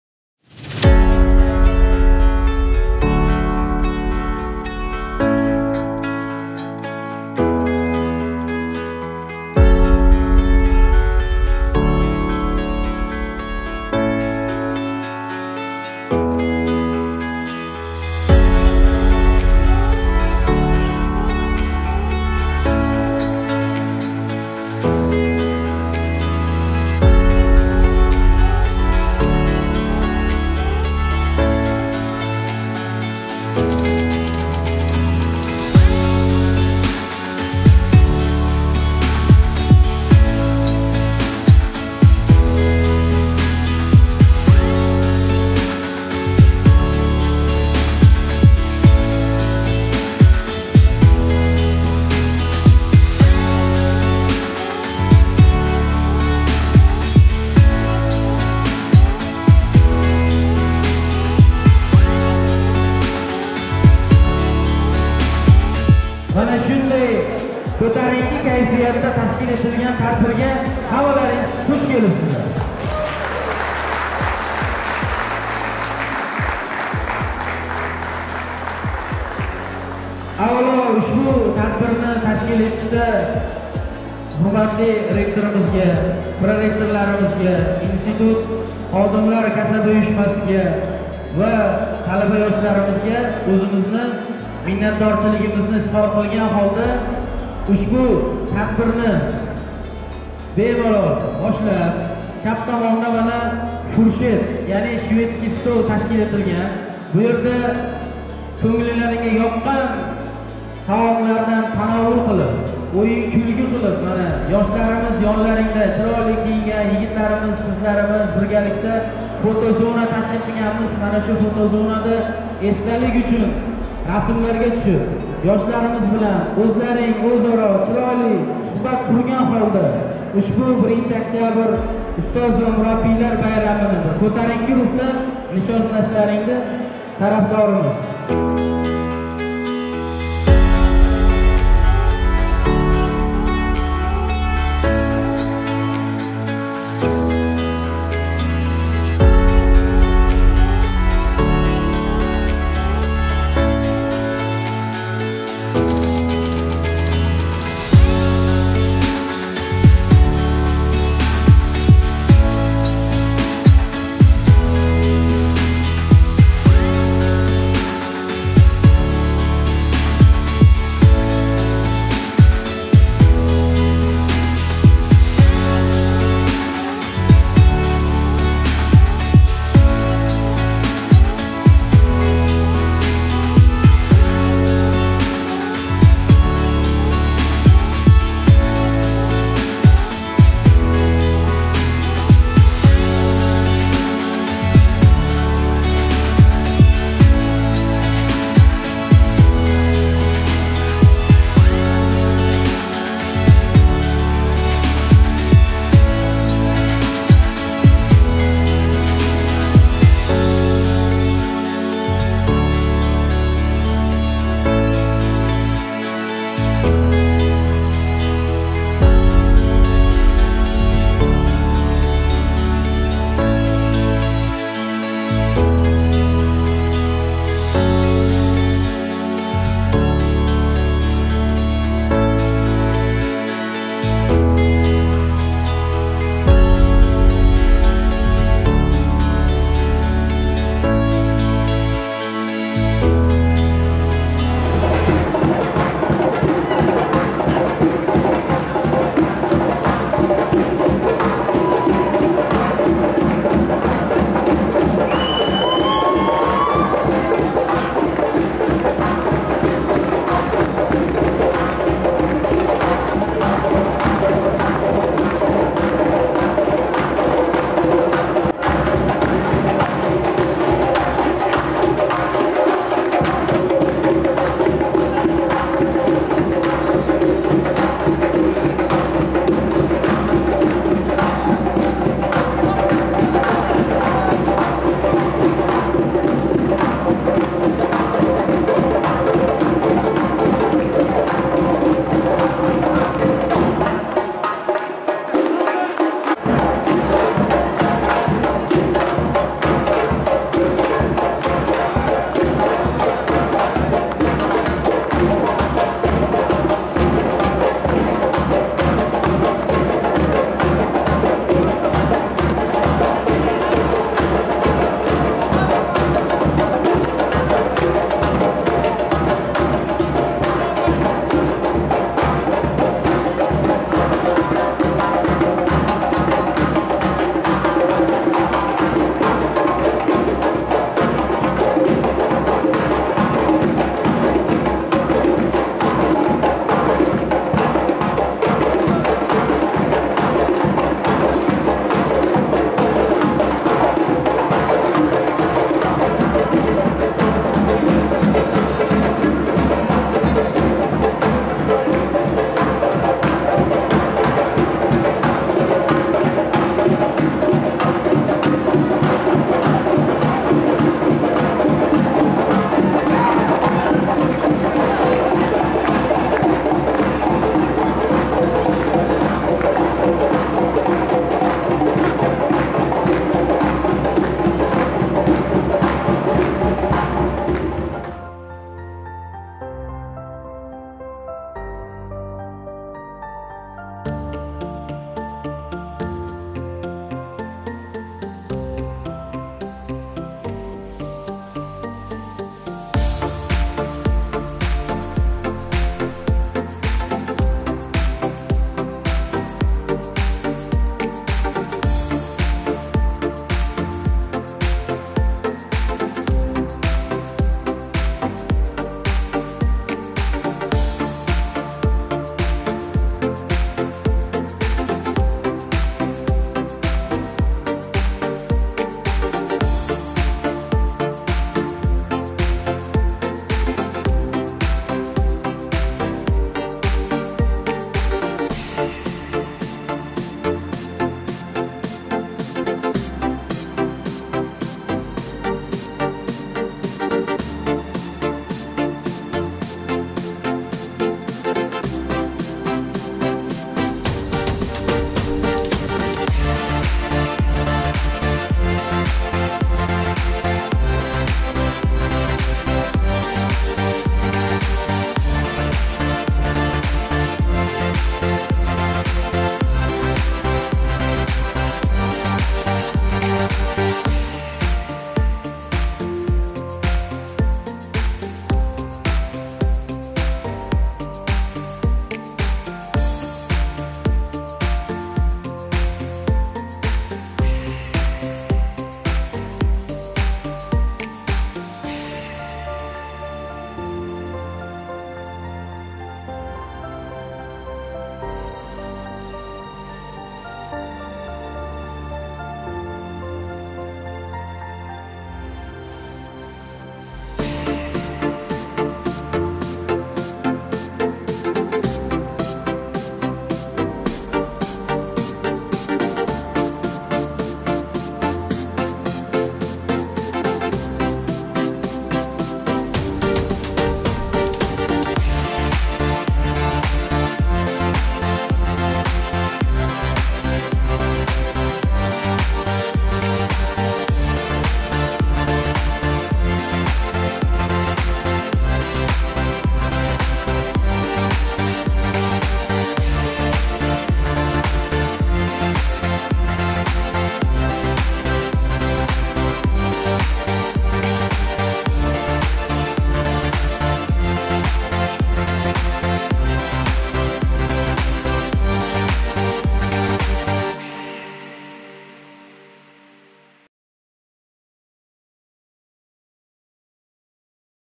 1-oktabr "Ustoz va murabbiylar kuni" bayram shukuhi
Farg'ona politexnika institutida muhtaram ustozlarga bag'ishlangan 1-oktabr "Ustoz va murabbiylar kuni" bayram shukuhi ko'tarinki ruhida tashkil etildi.
Ajoyib bayramona nozi-ne'matlar dasturxon ko'rkini bezadi. Shu bilan birqatorda tadbir davomida ko'ngillarni yangratuvchi kuy-navolar yangradi.

Ustozlarga berilgan e'tibor va tashkillangan bayram uchun Ustozlarimiz cheksiz minnatdorchilik bildirishdi.
Yurtimiz ravnaqi uchun kerakli kadrlar tayorlab kelayotgan va o'z kasbiga muhabbat bilan yondashgan ustozlarimizga katta raxmat!